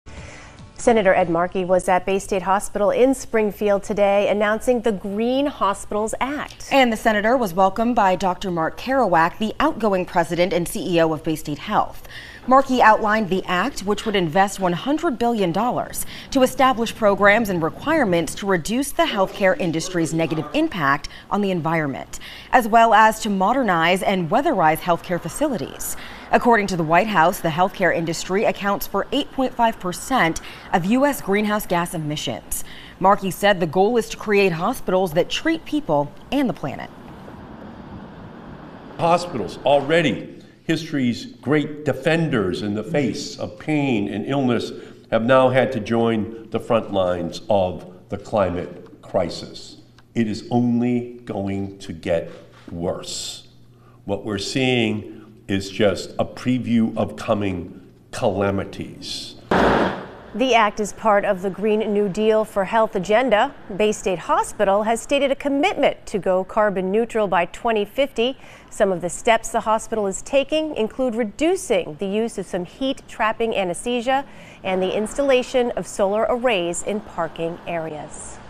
Climate change improvements for health care centers announced in Springfield
Senator Edward Markey held a news conference in Springfield to announce new legislation to help reduce climate change in health care centers.